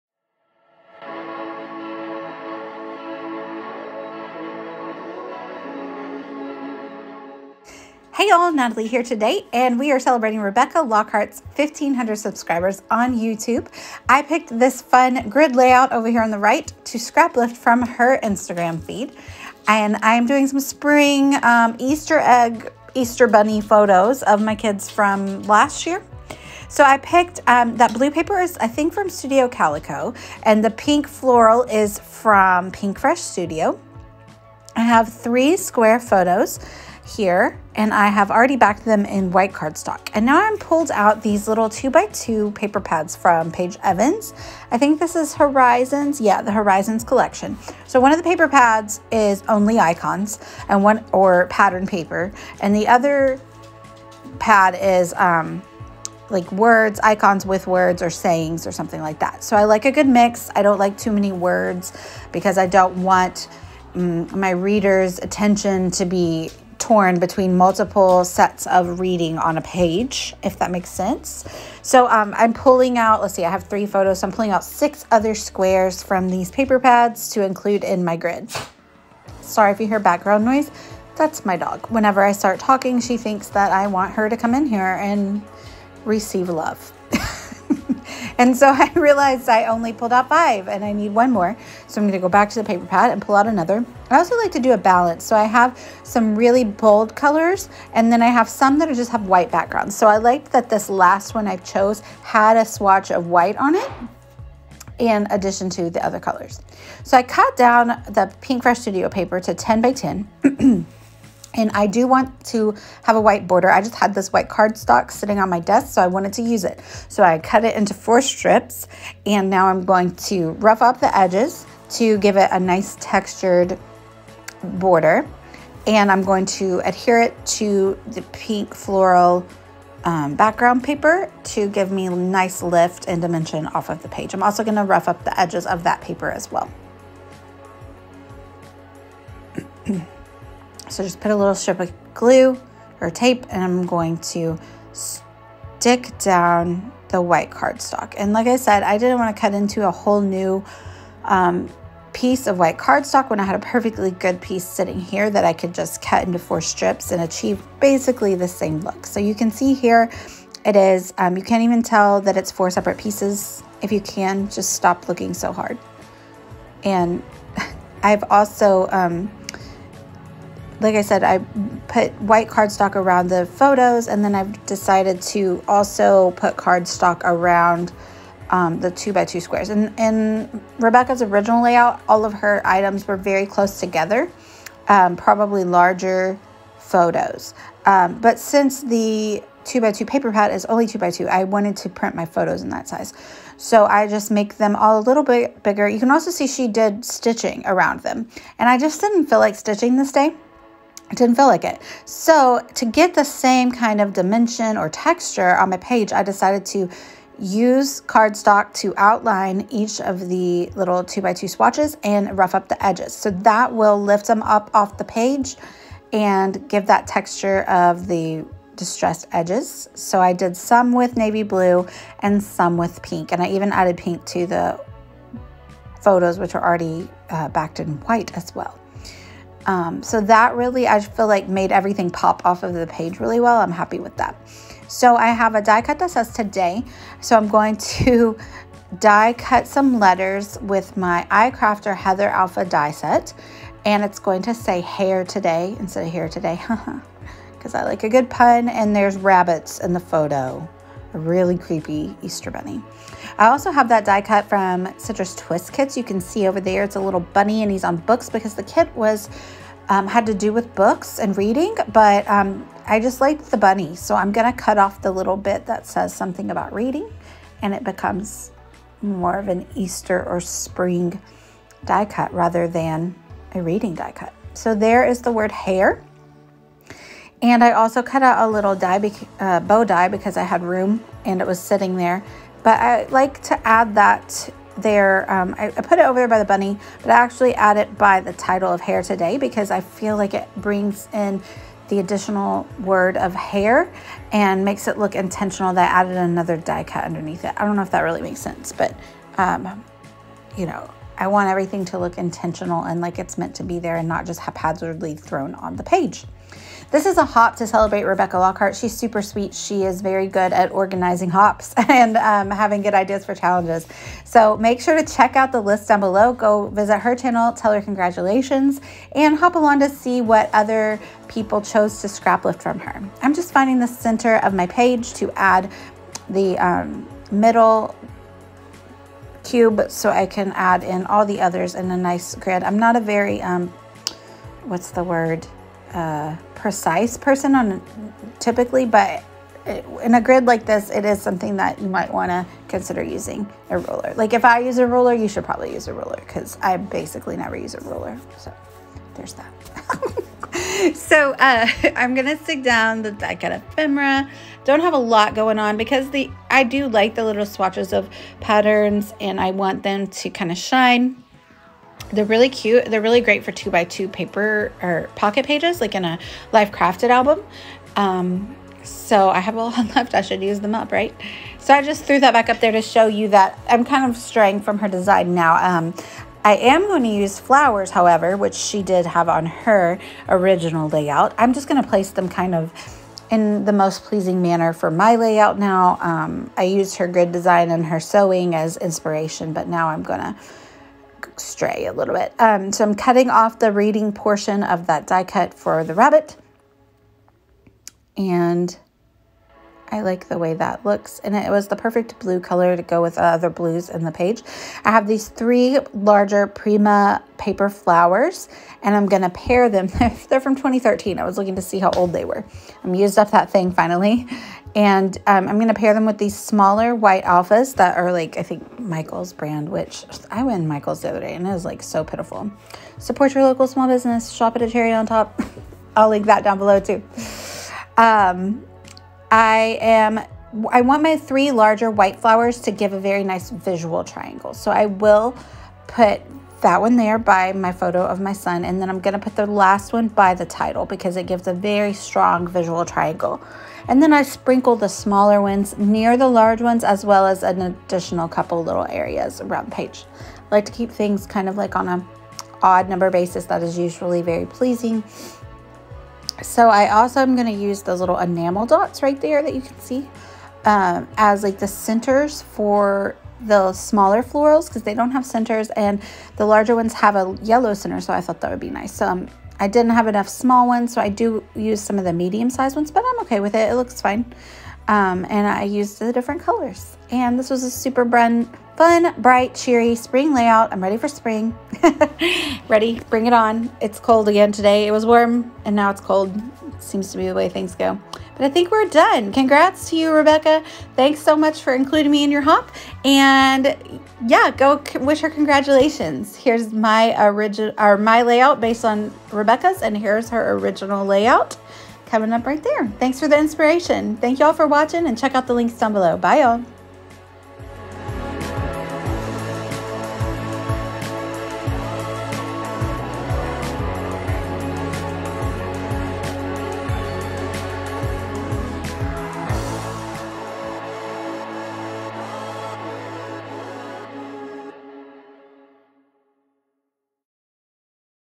Hare Today - celebrating Rebecca Lockhart!
Congrats to @RebeccaLockhartScraps for reaching 1500 subbies on YouTube!!

Hop along to help Rebecca celebrate!

I hope this is ok. Here’s a copy of the update links. I’ve added Crystal, Desiree, Amanda & Natalie.